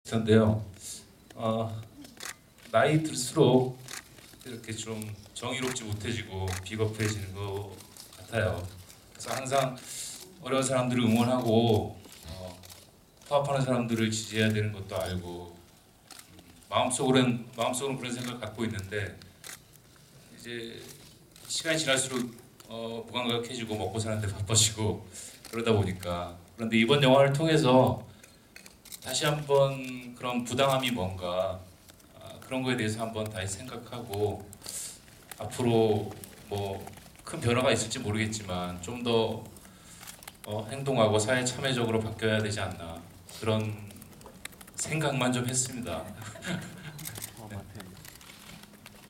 카트 이승준 "사측 인사로 출연한다"
2014년 10월 22일(수) 서울 자양동 롯데시네마 건대점에서 열린 영화 '카트'기자시사회&간담회 현장. 부지영 감독, 염정아, 문정희, 김영애, 김강우, 도경수, 황정민, 천우희, 이승준 출연의 영화 '카트'는 11월 13일 개봉된다.